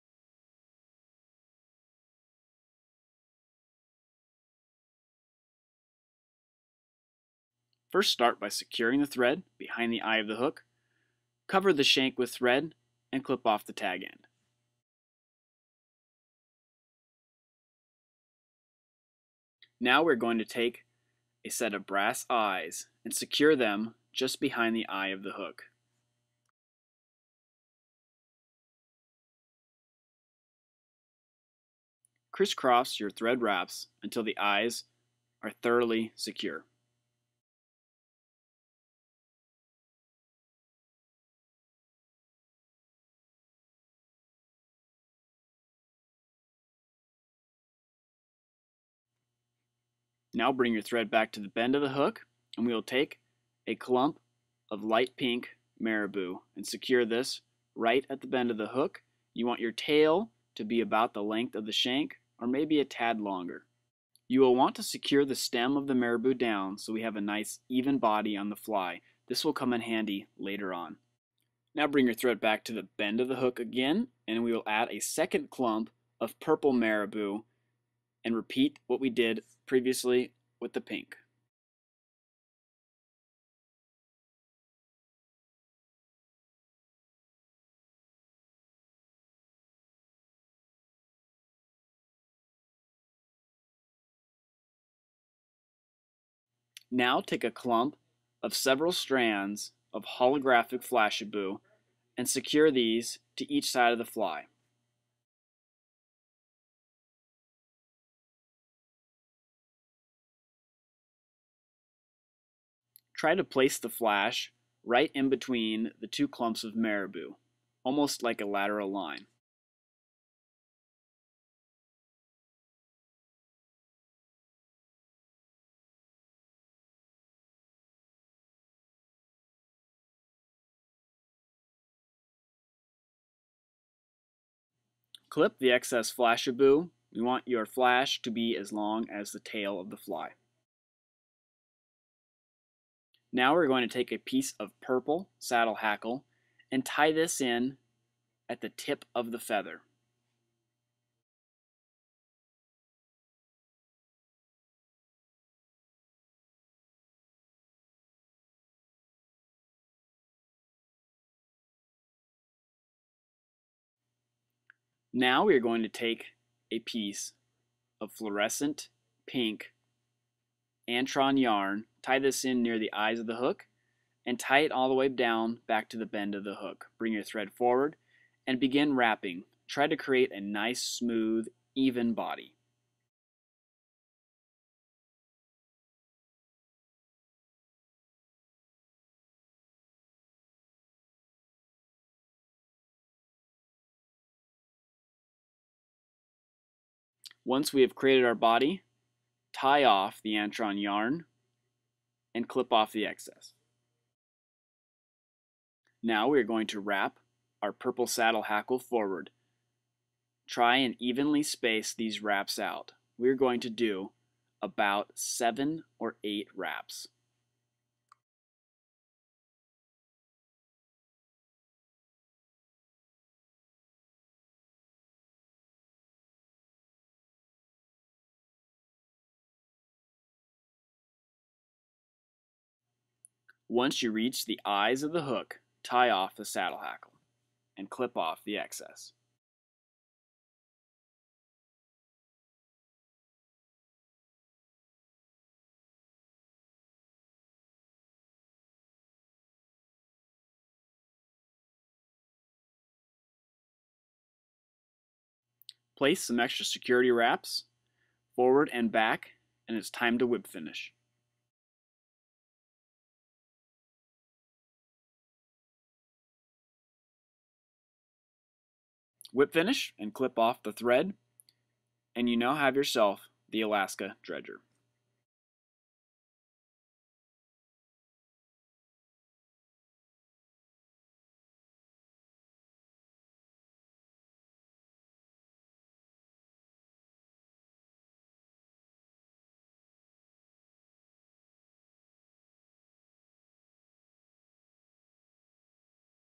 Steelhead Dredger Fly
This video will walk you through the steps on how to tie a Steelhead Dredger Fly